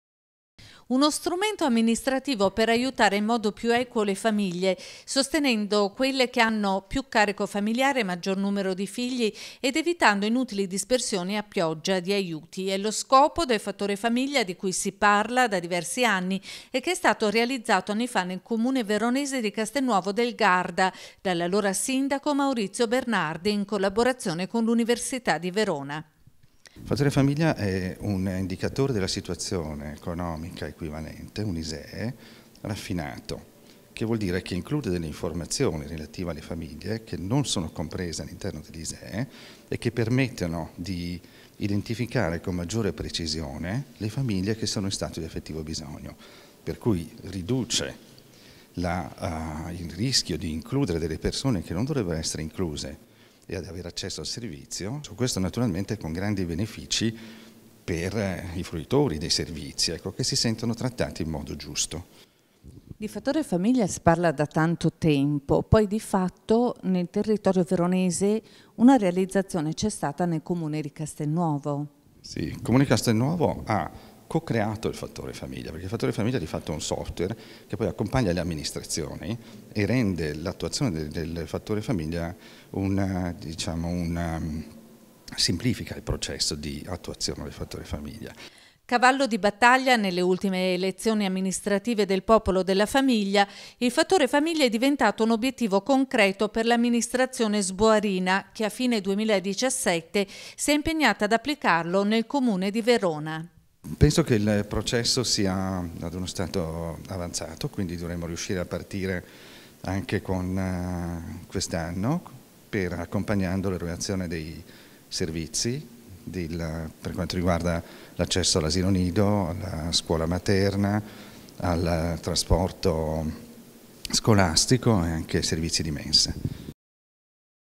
Aiuti più equi con il fattore famiglia
Politiche familiari più eque con il fattore famiglia. L’impegno per Verona.
FEDERICO PERALI -  Doc. Politica Economica – Università di Verona